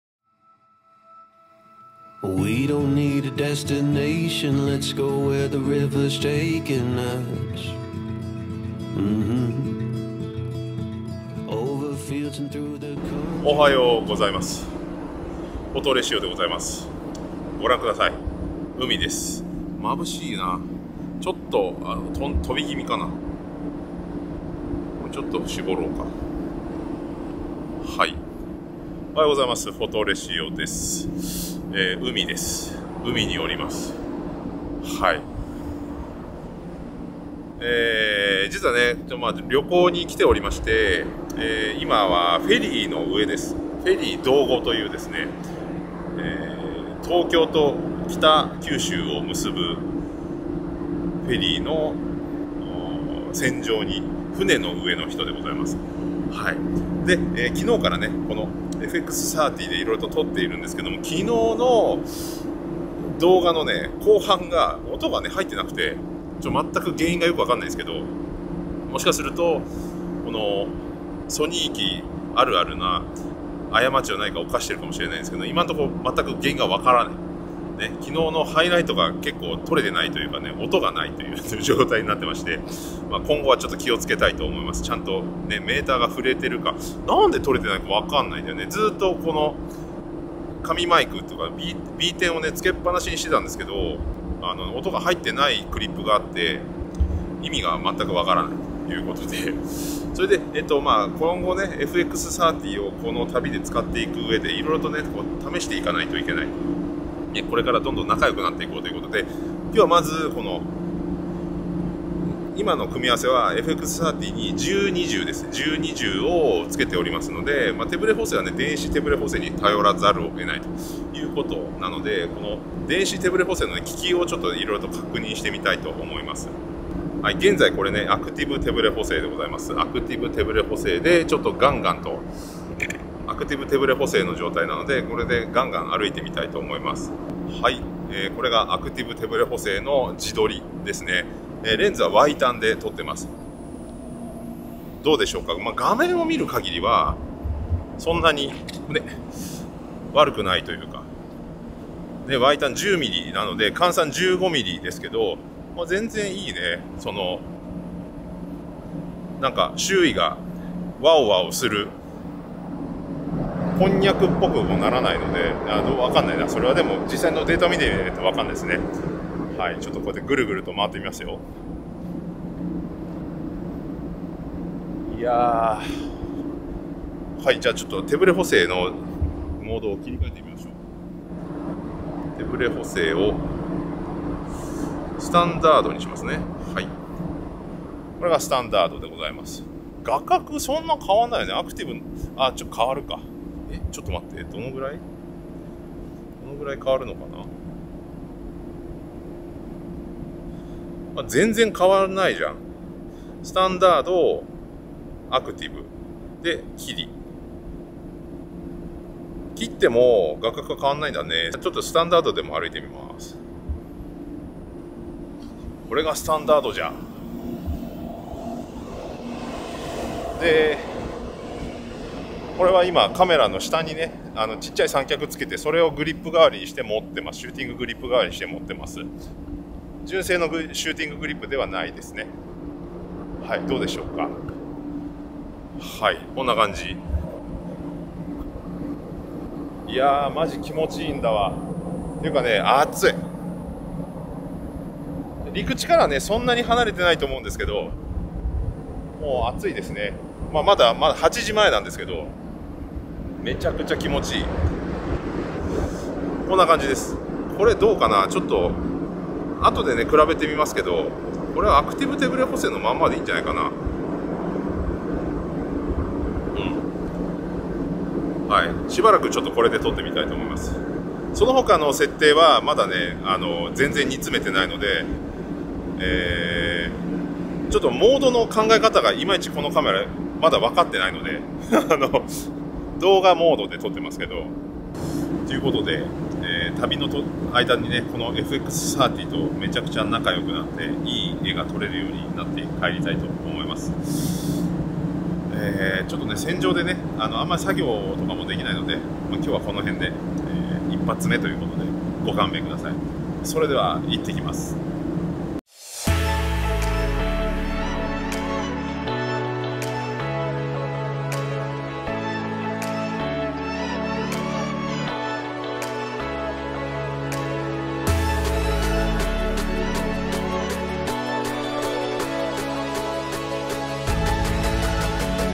130「旅先でFX30の手ぶれ補正を試す￼。」
買ったばかりのFX30と旅に出ております￼。
今日はフェリーの上で手ぶれ補正を試しました￼。

FX30 + E PZ 10-20 F4 + ECM-B10

で撮影しています￼。手ぶれ補正イマイチって聞いてたけどVlog的運用も全然いけますね￼￼。